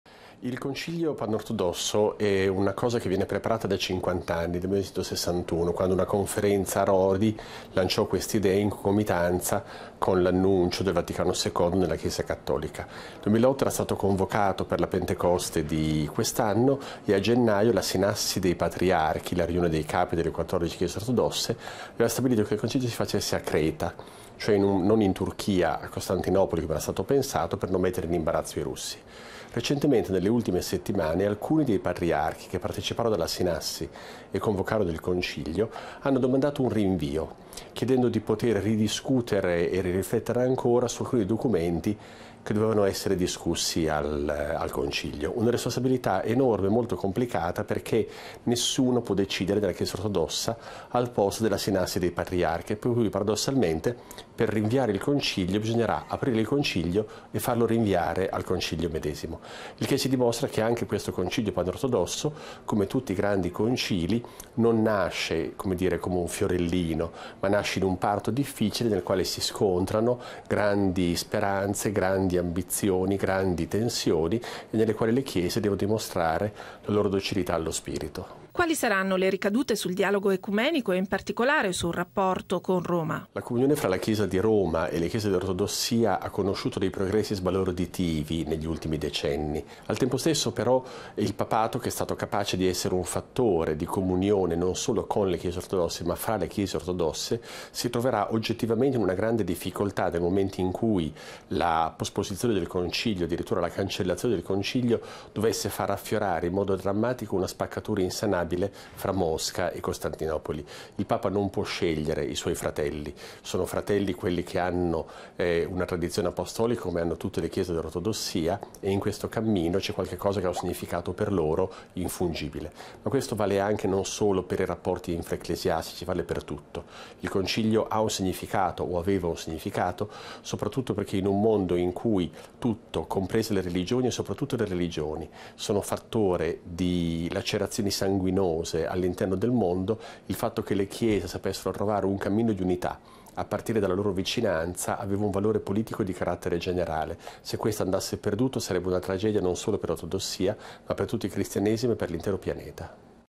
Concilio panortodosso: l'intervista allo storico Alberto Melloni
Torniamo sulla notizia anticipata ieri, niente da fare per una presenza piena al Concilio panortodosso. A gelare ogni speranza di vedere riunite per la prima volta nella storia tutte le  quattordici chiese ortodosse il comunicato con cui il Patriarcato di Mosca ieri ha chiesto formalmente un rinvio dell'assise.  50 anni di preparazione dunque non sono bastati per mettere insieme tutte le anime dell'ortodossia. Abbiamo chiesto allo storico Alberto Melloni cosa è accaduto e quali sono le conseguenze sul piano ecumenico. Servizio di Cristiana Caricato.